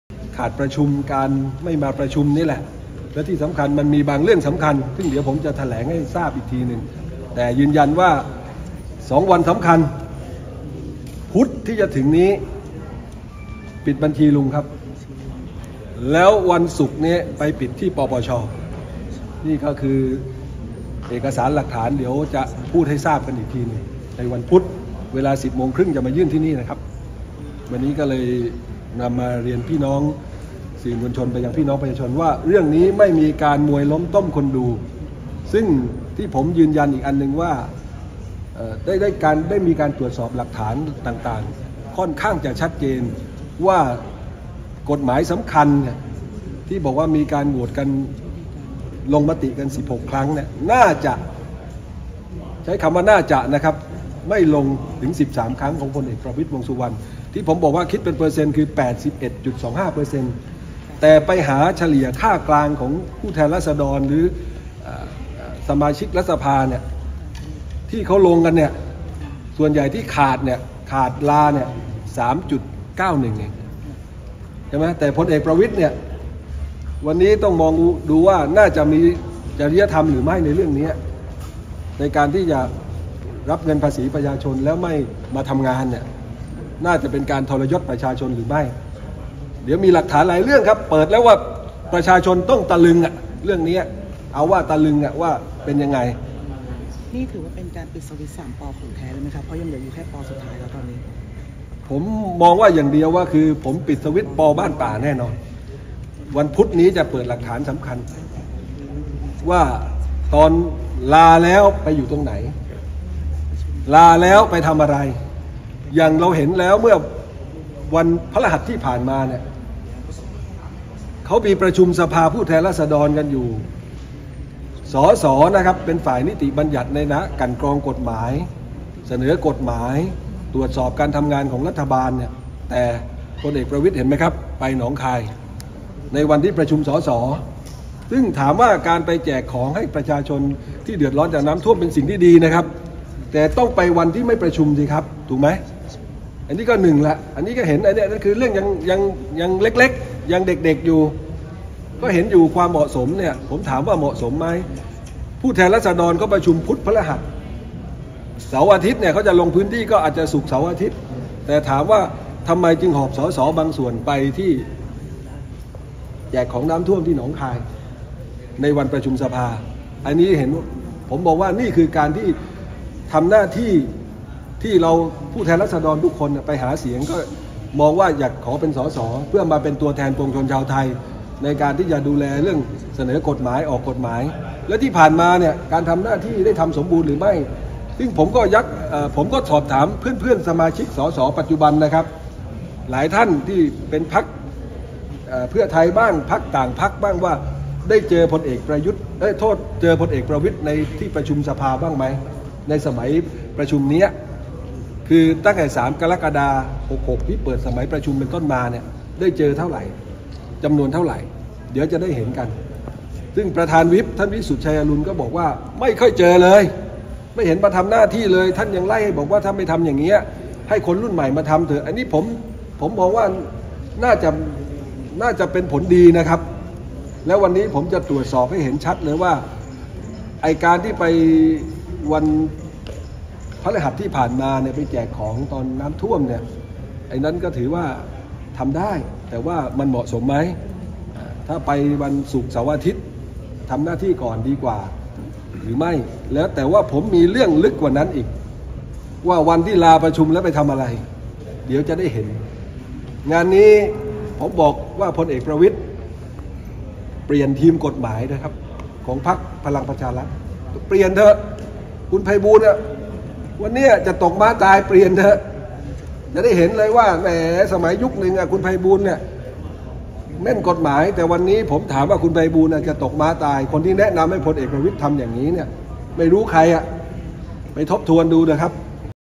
’เด็จพี่‘ โวพุธนี้ปิดบัญชี ’ลุงป้อม‘ ส่งไม้ต่อ ป.ป.ช.สอบจริยธรรม ยันไม่มีมวยล้มต้มคนดู
’เด็จพี่‘ โวหลักฐานเพียบ พุธนี้ปิดบัญชี ’ลุงป้อม‘ ได้แน่ พร้อมส่งไม้ต่อ ป.ป.ช.สอบจริยธรรม ลั่นงานนี้ไม่มีมวยล้มต้มคนดู ไล่เปลี่ยนทีมกฎหมาย อ้าง ‘ไพบูลย์’ หมดกึ๋นตกม้าตาย